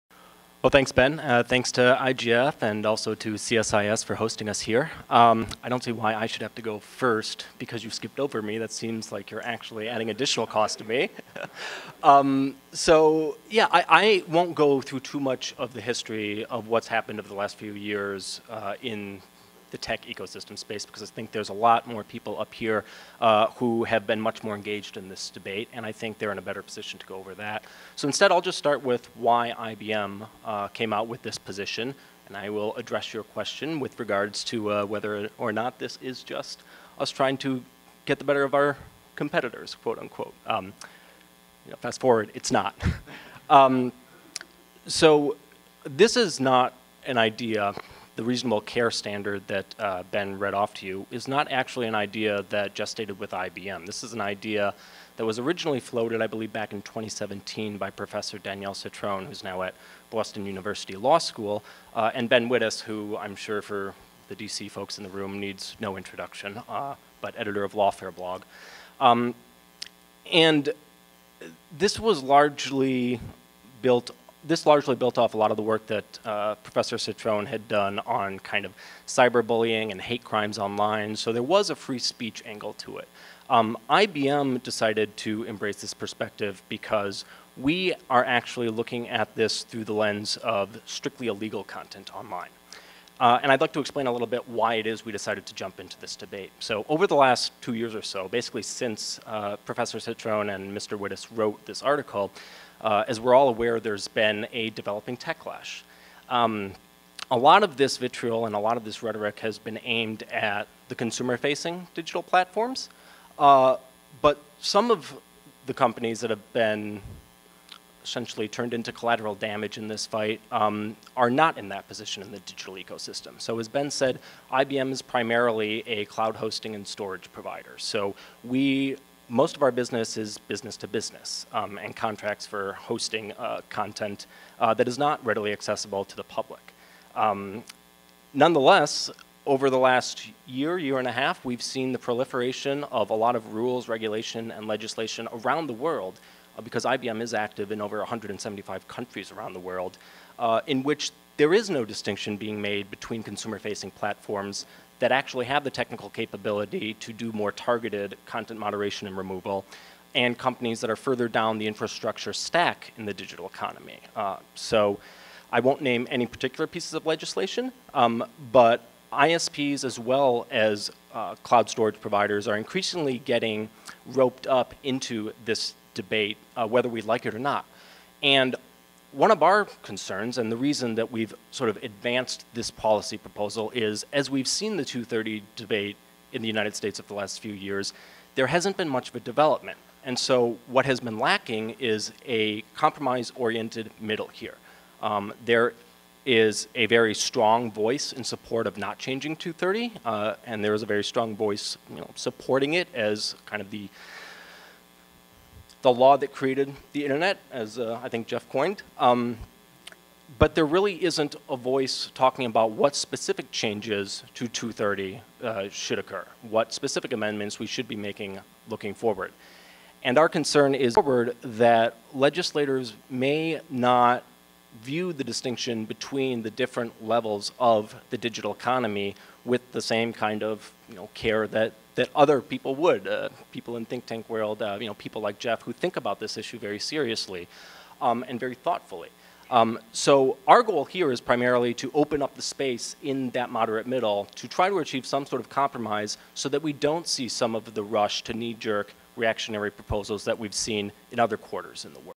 Ryan Hagemann talks of supporting changes to Section 230
Ryan Hagemann, Technology Policy Executive at IBM, says people talk about the concept of changing or not changing Section 230, but not specifically what changes should be made.